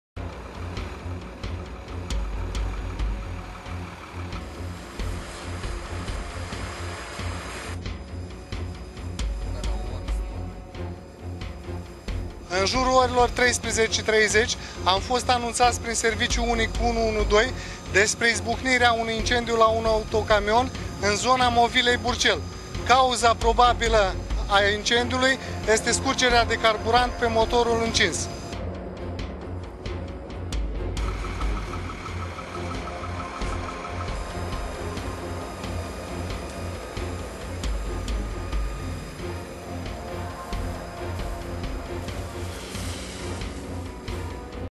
incendiu masina 26 09 2009
Un sucevean a ramas fara masina in zona Movila lui Burcel din judetul Vaslui dupa ce autocamionul a luat foc. Soferul transporta grau, luat la schimb pe cartofi, din zona Barladului iar probabil in urma unei scurgeri de combustibil pe motorul incins, masina a luat foc.